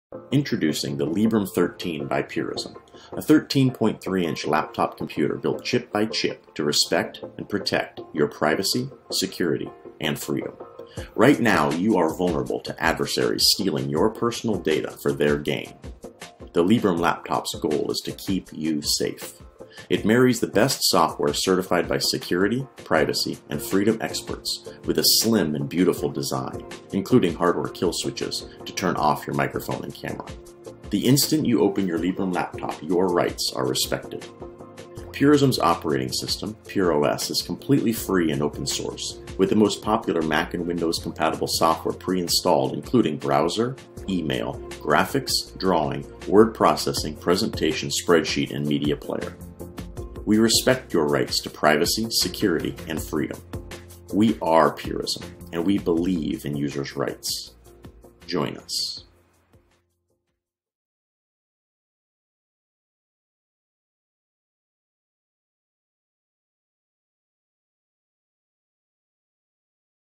Purism's Librem 13 Linux Laptop Is Sleek, Private and Secure
The combination of custom-made hardware paired with a tweaked Linux OS makes the Librem laptop lineup a unique offering with several innovative security features not offered in any other computer.

The Librem line is a work in progress. The operating system just reached version 2.0 and comes preinstalled on the hardware built with the modified Linux kernel in mind.

LinuxInsider received one of the first available Librem 13-inch units for testing and review. Our hands-on testing shows the hardware/software combo is an impressive display of the power and finesse of Linux.

The Librem laptops are designed around beautiful and effective hardware. They are built chip by chip, application by application for privacy and security.